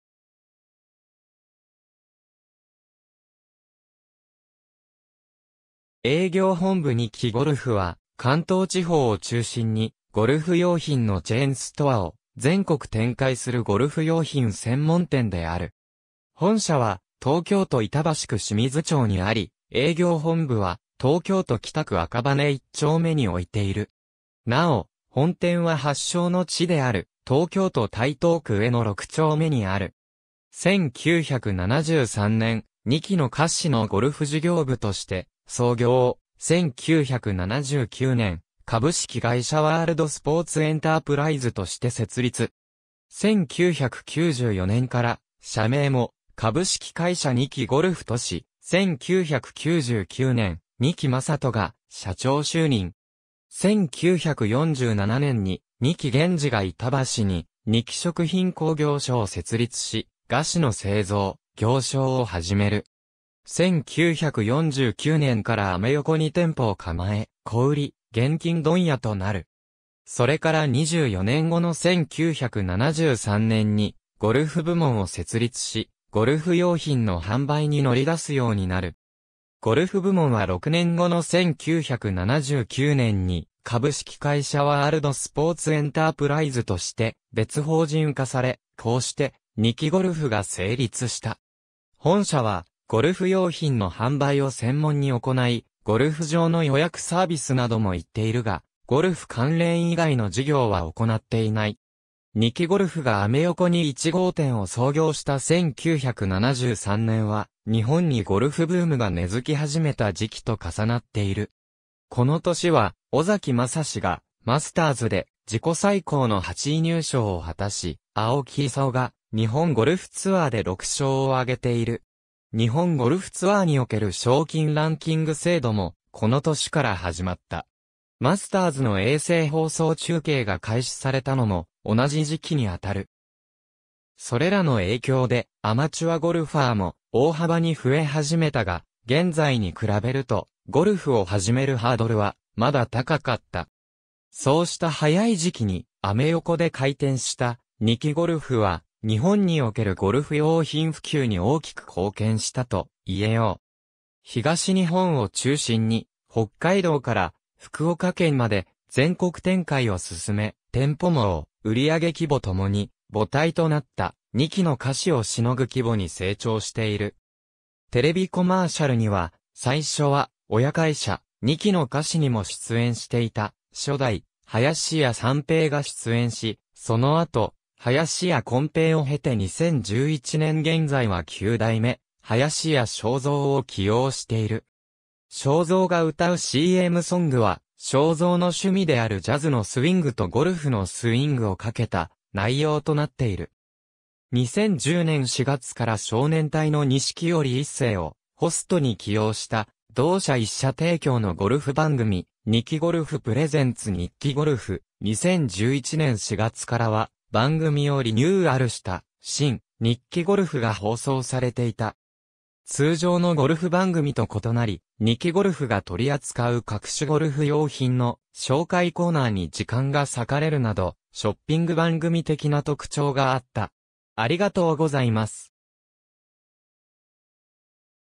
二木ゴルフ
二木ゴルフ（にきゴルフ）は、関東地方を中心にゴルフ用品のチェーンストアを全国展開するゴルフ用品専門店である。
本社は東京都板橋区清水町にあり、営業本部は東京都北区赤羽一丁目に置いている。
なお、本店は発祥の地である東京都台東区上野六丁目（アメ横）にある。
博多麦野店 1973年（昭和48年）「二木の菓子」のゴルフ事業部として創業、1979年（昭和54年）「株式会社ワールドスポーツエンタープライズ」（商標は「二木ゴルフ」）として設立。
1994年（平成6年）から社名も「株式会社二木ゴルフ」とし、1999年（平成11年）二木正人が社長就任。
1947年（昭和22年）に二木源治が板橋に「二木食品工業所」を設立し、菓子の製造・行商を始める。
1949年（昭和24年）からアメ横に店舗を構え、小売・現金問屋となる。
それから24年後の1973年（昭和48年）にゴルフ部門を設立し、ゴルフ用品の販売に乗り出すようになる。
ゴルフ部門は6年後の1979年（昭和54年）に「株式会社ワールドスポーツエンタープライズ」として別法人化され、こうして「二木ゴルフ」が成立した。
本社はゴルフ用品の販売を専門に行い、ゴルフ場の予約サービスなども行っているが、ゴルフ関連以外の事業は行っていない。
二木ゴルフがアメ横に1号店を創業した1973年（昭和48年）は、日本にゴルフブームが根づき始めた時期と重なっている。
この年は尾崎将司がマスターズで自己最高の8位入賞を果たし、青木功が日本ゴルフツアーで6勝を挙げている。
日本ゴルフツアーにおける賞金ランキング制度も、この年から始まった。
マスターズの衛星放送中継が開始されたのも同じ時期にあたる。
それらの影響で、アマチュアゴルファーも大幅に増え始めたが、現在に比べるとゴルフを始めるハードルはまだ高かった。
そうした早い時期にアメ横で開店した二木ゴルフは、日本におけるゴルフ用品普及に大きく貢献したといえよう。
東日本を中心に、北海道から福岡県まで全国展開を進め、店舗網、売り上げ規模ともに、母体となった二木の菓子を凌ぐ規模に成長している。
テレビコマーシャルには、最初は親会社・二木の菓子にも出演していた初代林家三平が出演し、その後林家こん平を経て2011年現在は9代目林家正蔵を起用している（林家こぶ平時代に収録、長年にわたり1993年に収録された旧バージョンを放映していたが、2002年に撮り直し、現在はその新バージョンを使用）。
正蔵が歌うCMソングは、正蔵の趣味であるジャズのスウィングとゴルフのスイングをかけた内容となっている。
2010年4月から少年隊の錦織一清をホストに起用した、同社一社提供のゴルフ番組『二木ゴルフ Presents ニッキゴルフ』（TOKYO MX）、2011年4月からは、番組をリニューアルした『真・ニッキゴルフ』が放送されていた。
通常のゴルフ番組と異なり、二木ゴルフが取り扱う各種ゴルフ用品の紹介コーナーに時間が割かれるなどショッピング番組的な特徴があった。